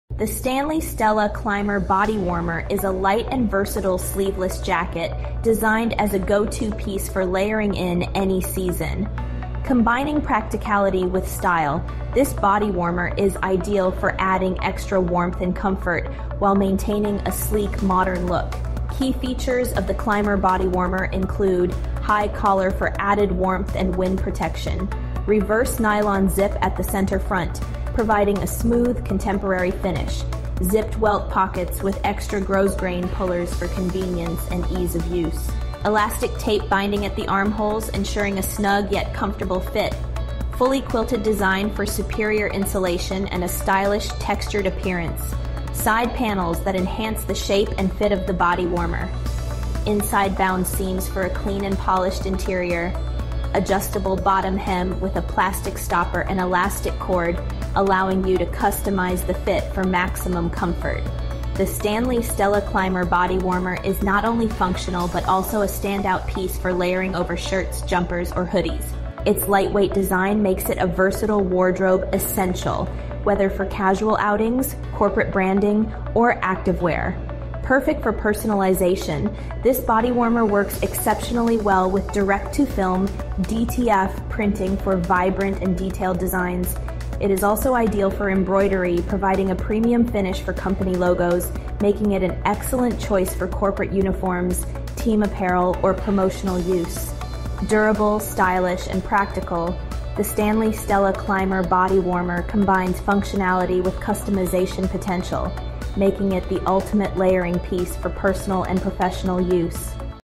Stanley Stella Climber Bodywarmer
The Stanley/Stella Climber Bodywarmer is a light and versatile sleeveless jacket, designed as a go-to piece for layering in any season. Combining practicality with style, this bodywarmer is ideal for adding extra warmth and comfort while maintaining a sleek, modern look.

Key features of the Climber Bodywarmer include:

High collar for added warmth and wind protection.
Reverse nylon zip at the centre front, providing a smooth, contemporary finish.
Zipped welt pockets with extra grosgrain pullers for convenience and ease of use.
Elastic tape binding at the armholes, ensuring a snug yet comfortable fit.
Fully quilted design for superior insulation and a stylish, textured appearance.
Side panels that enhance the shape and fit of the bodywarmer.
Inside bound seams for a clean and polished interior.
Adjustable bottom hem with a plastic stopper and elastic cord, allowing you to customise the fit for maximum comfort.
The Stanley/Stella Climber Bodywarmer is not only functional but also a standout piece for layering over shirts, jumpers, or hoodies. Its lightweight design makes it a versatile wardrobe essential, whether for casual outings, corporate branding, or activewear.

Perfect for personalisation, this bodywarmer works exceptionally well with direct-to-film (DTF) printing for vibrant and detailed designs. It is also ideal for embroidery, providing a premium finish for company logos, making it an excellent choice for corporate uniforms, team apparel, or promotional use.

Durable, stylish, and practical, the Stanley/Stella Climber Bodywarmer combines functionality with customisation potential, making it the ultimate layering piece for personal and professional use.